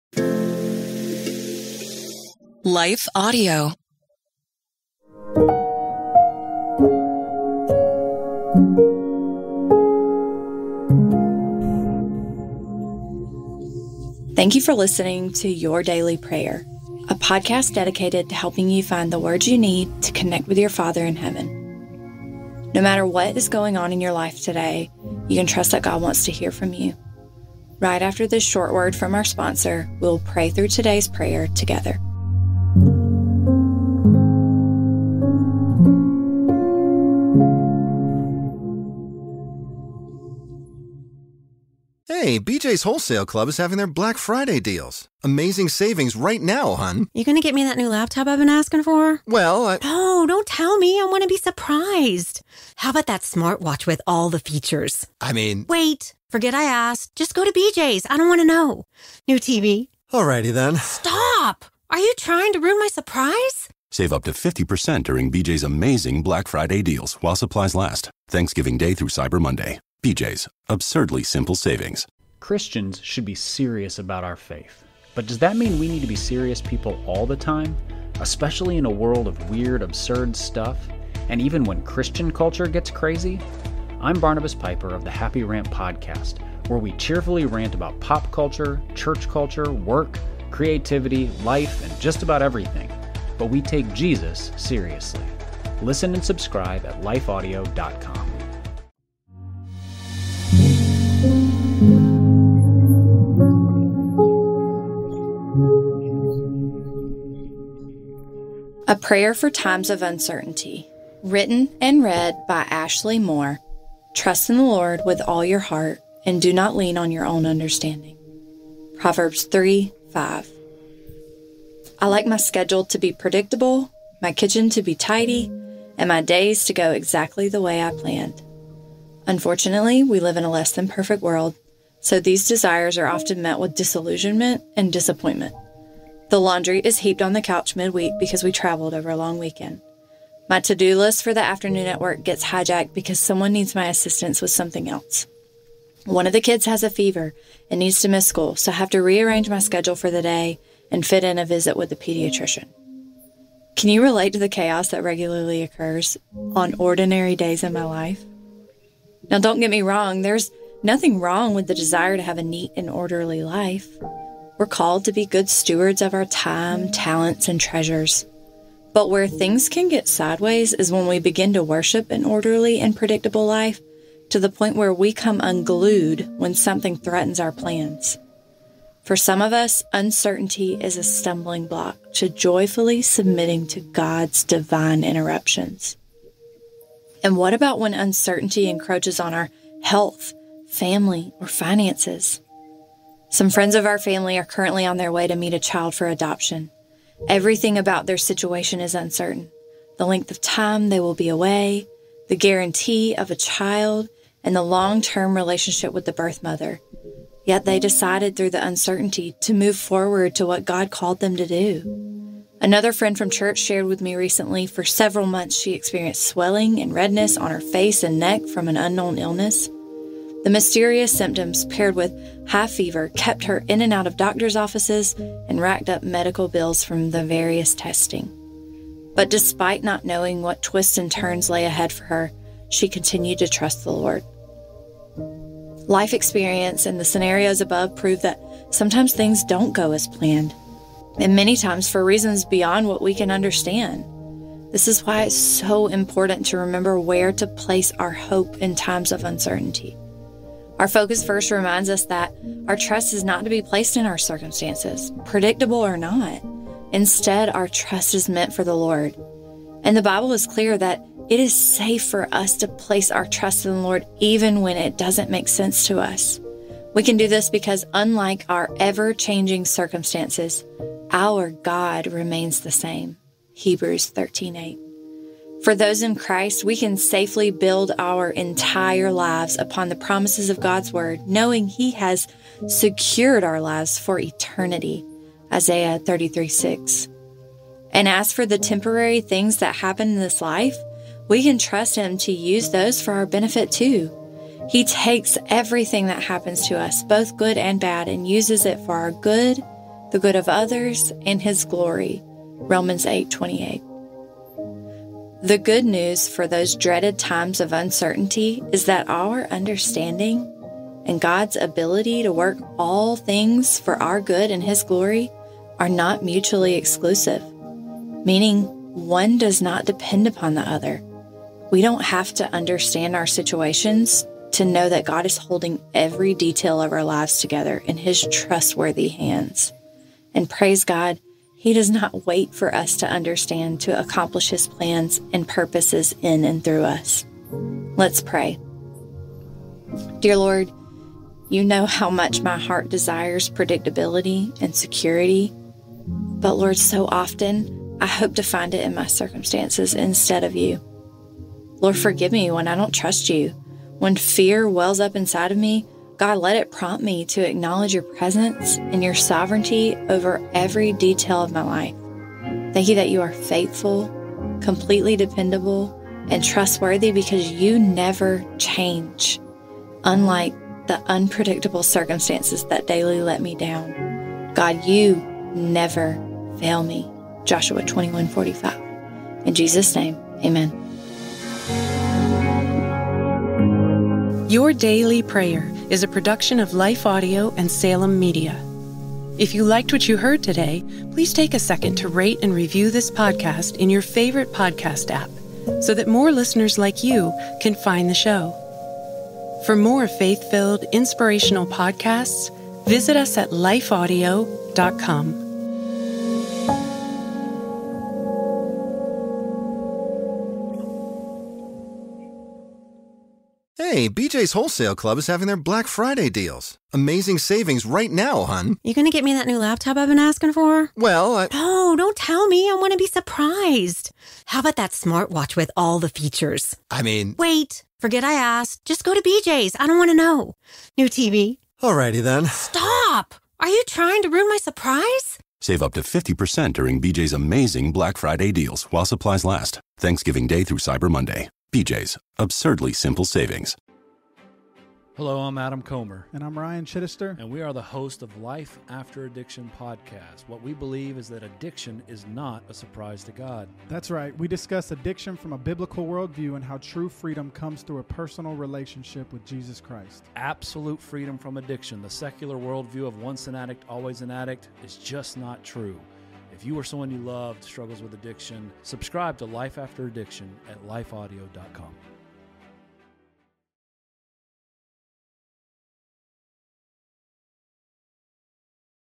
A Prayer for Times of Uncertainty
We are called to be good stewards of our time, talents, and treasures. But where things can get sideways is when we begin to worship an orderly and predictable life to the point where we come unglued when something threatens our plans. For some of us, uncertainty is a stumbling block to joyfully submitting to God’s divine interruptions. - Ashley MooreSUBSCRIBE to our sister podcasts:The Crosswalk Devotional: Daily Bible Verse: